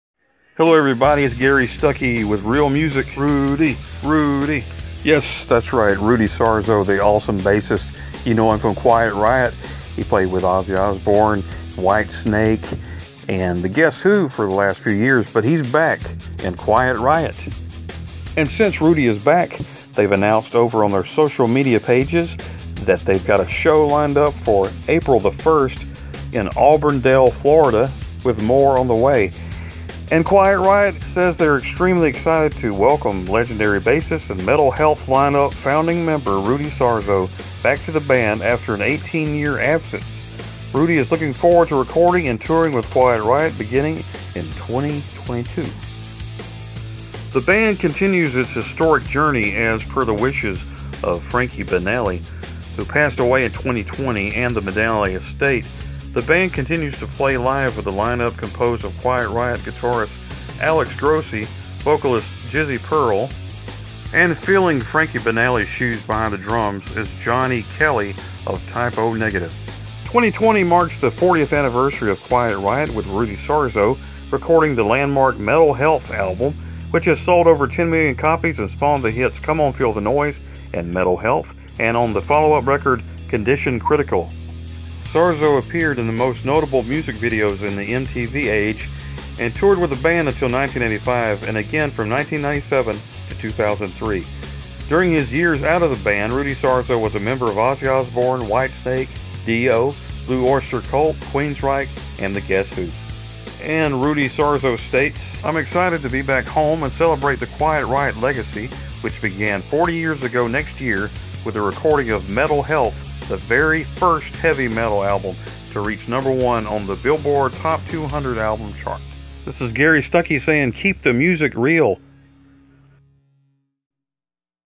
Rudy Sarzo Back In Quiet Riot
A new concert date announced with more on the way in 2022.